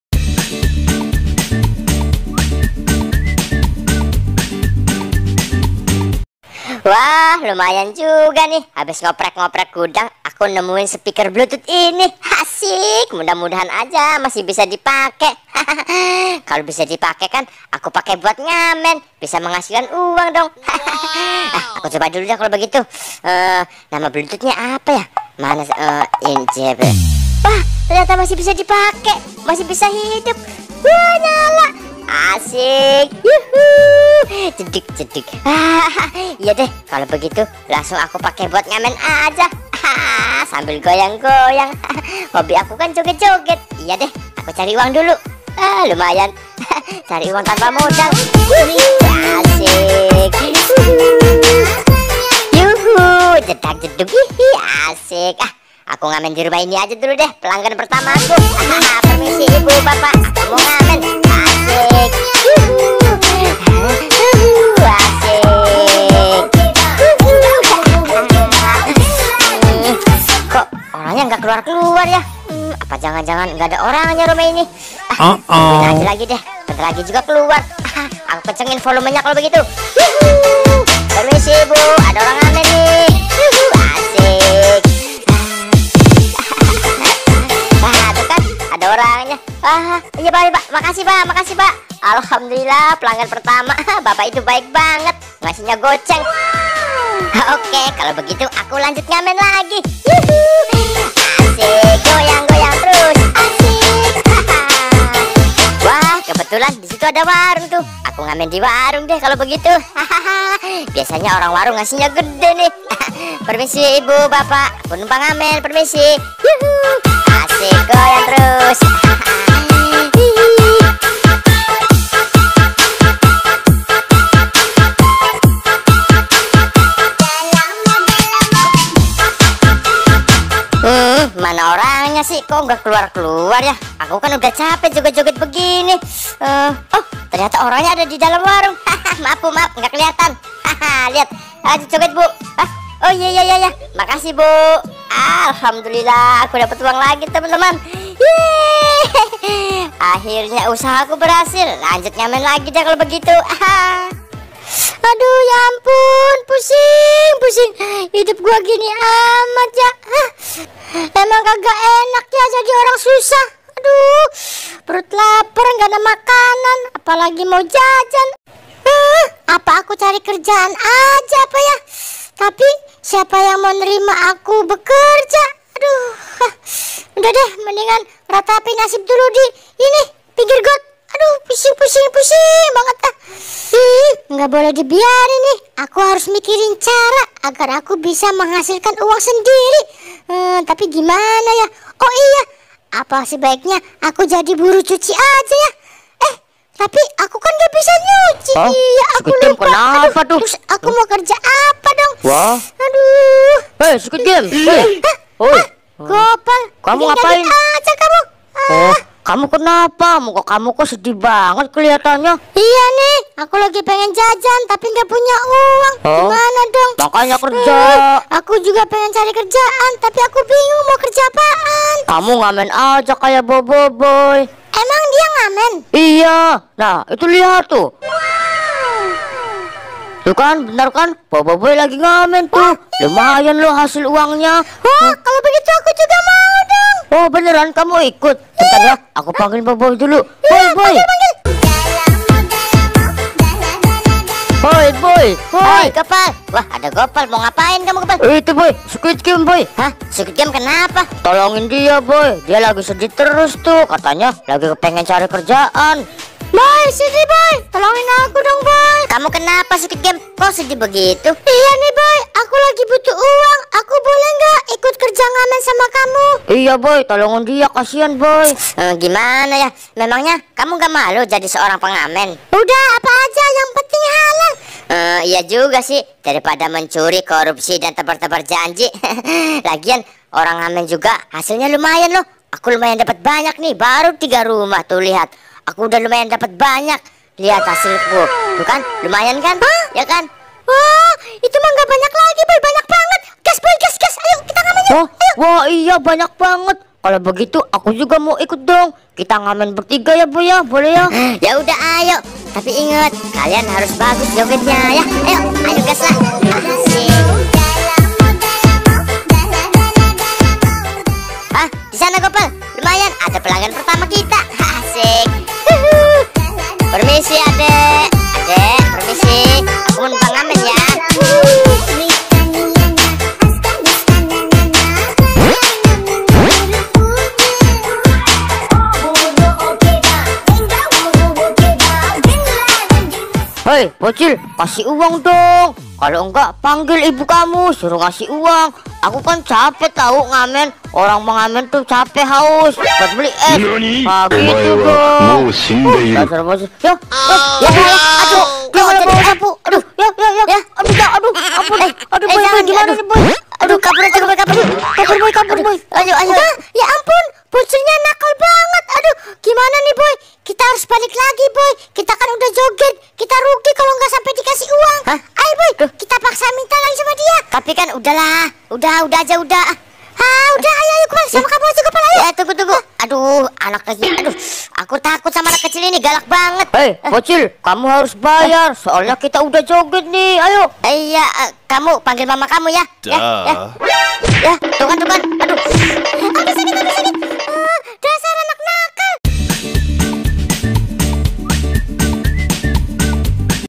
BADUT BOBOIBOY BADUT SQUIDGAME JADI TUKANG NGAMEN DIUSIR BOCIL DIPUKUL PAKE SAPU
BADUT BOBOIBOY BADUT SQUIDGAME JADI TUKANG NGAMEN DIUSIR BOCIL DIPUKUL PAKE SAPU BOCIL GALAK BANGET